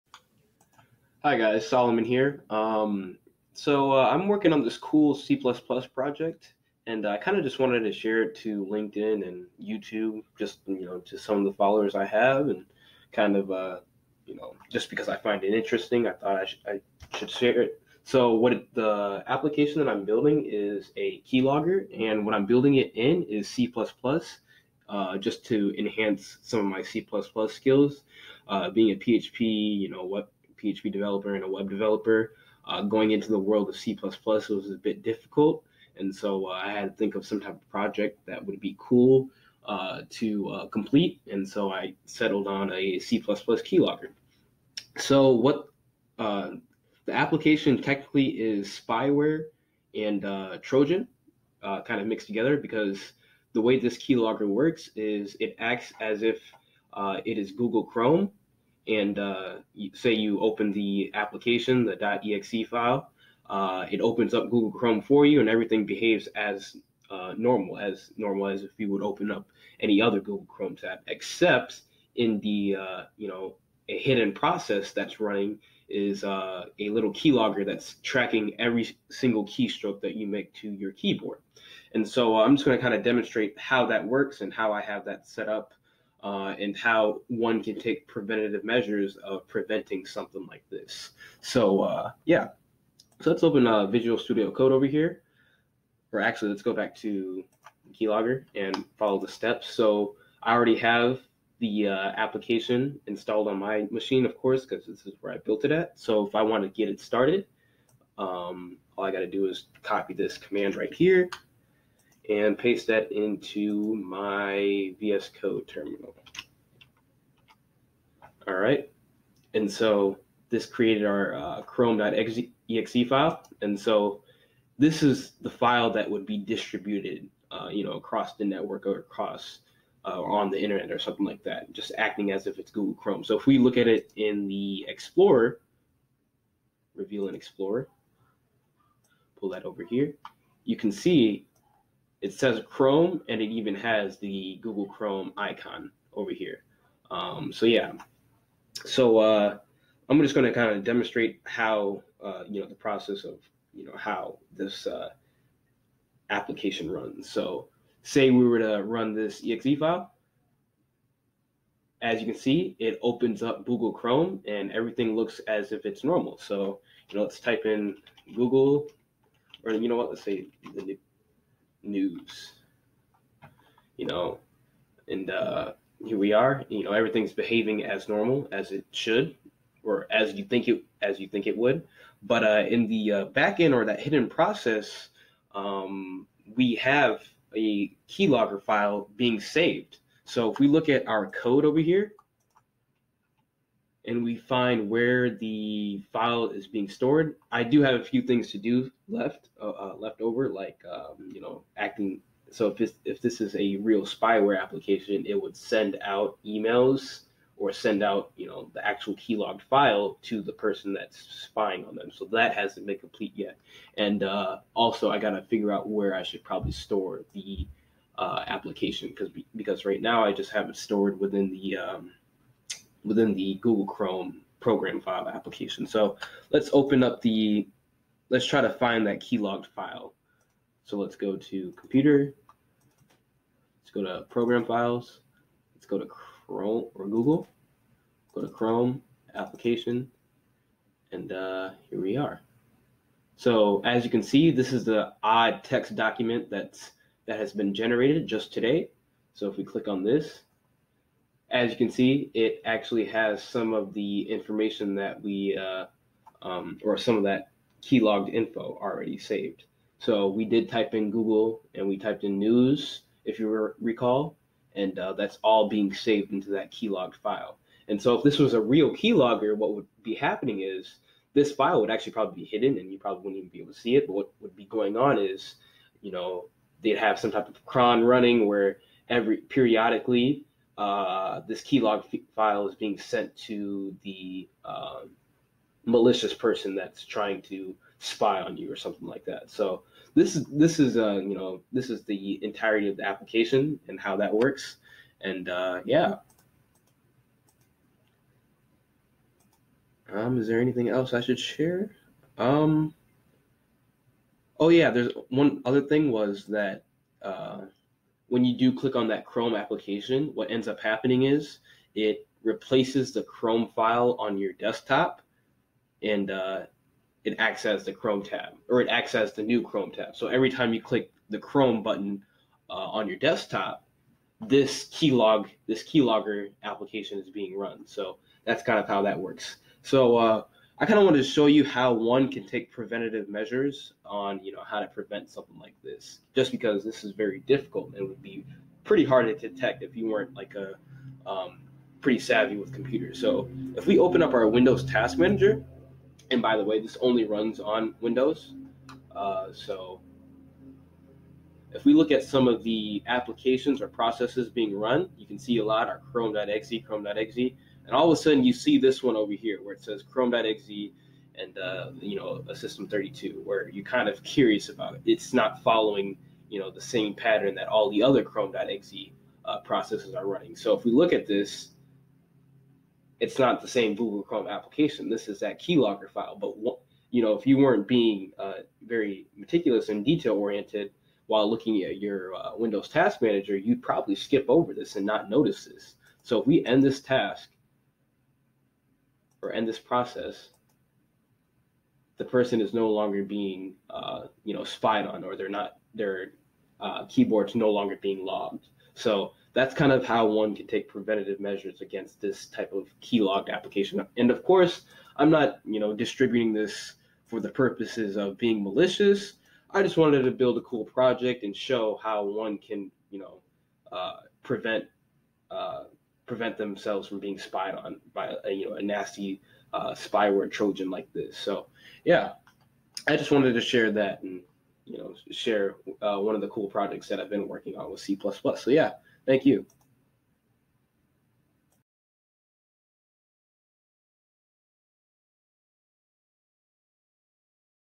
C++ Keylogger Demo
Demonstration of the keylogger i built with C++ This Phishing Keylogger is a Trojan that downloads an executable file to windows which then creates a shortcut icon on your desktop and disguises itself as your default browser. Whenever you click the shortcut icon, the keylogger will create a hidden file that will log everything from your keyboard and then open your default browser for you. As you are surfing the web everything your typing is saved in the key logged file, once you close the application the keylogger executable will stop and save the hidden file. Everytime you open a new browser the process repeats itself. I built the application with C++.